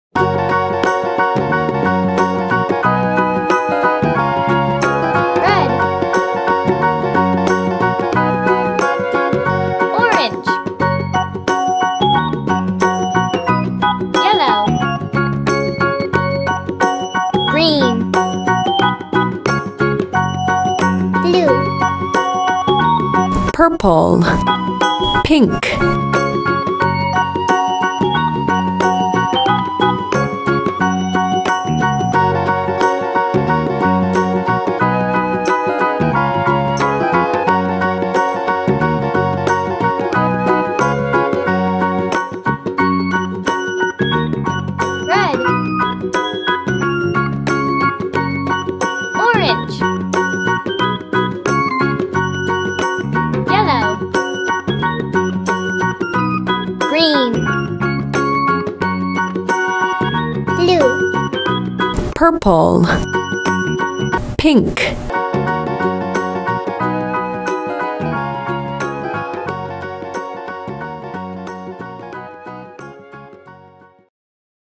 Spelling colors in rainbow
Educational video. Spelling colors in rainbow. Learn colors the fun way.